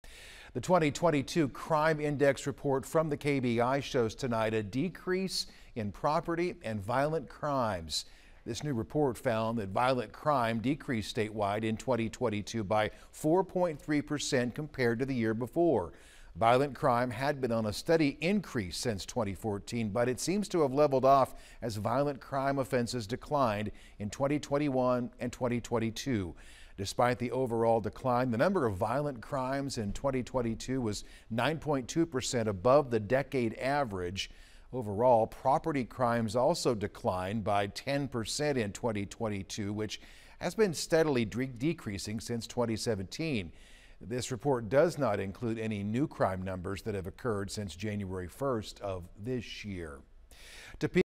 Kansas sees decrease in crime statewide according to 2022 report
Live at Five